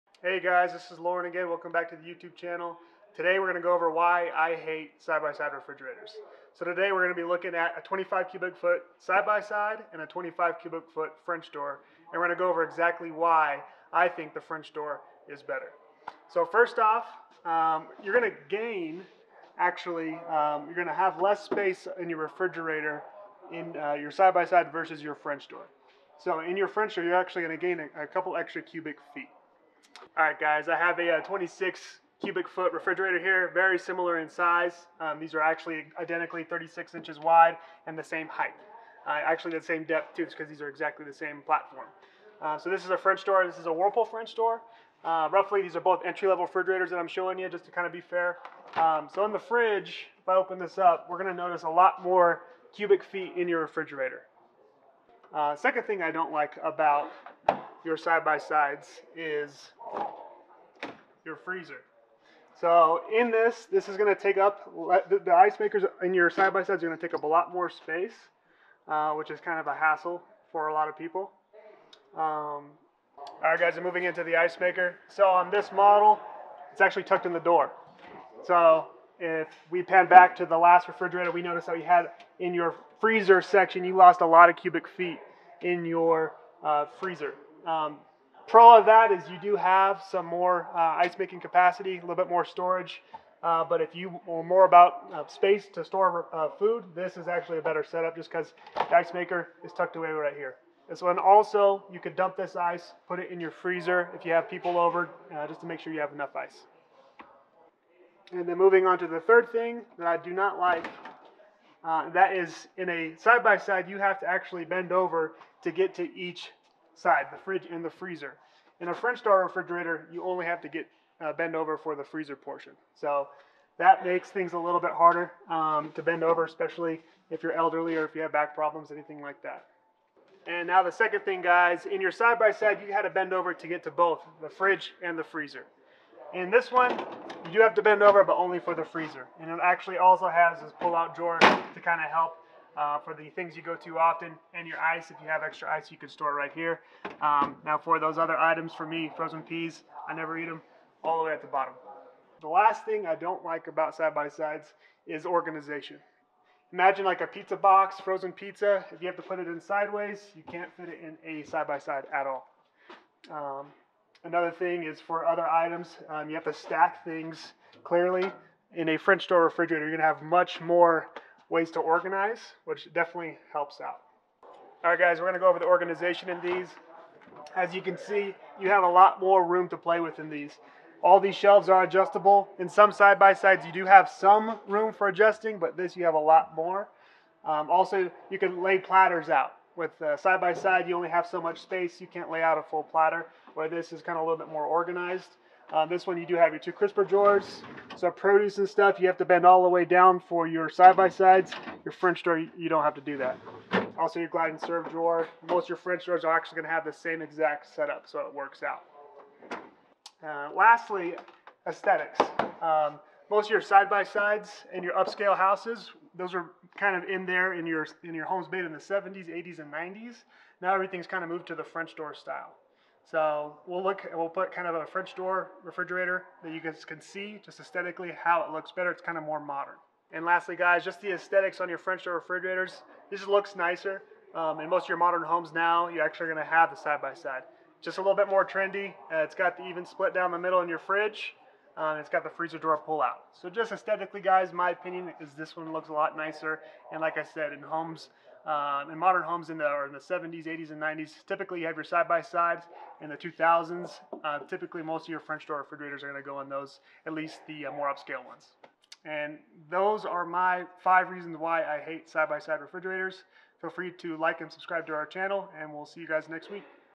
Top 5 Things I Hate About Side by Side Refrigerators
Side by side VS french door refrigerators! Loren goes over his reasons why he hates side by side refrigerators and prefers french doors.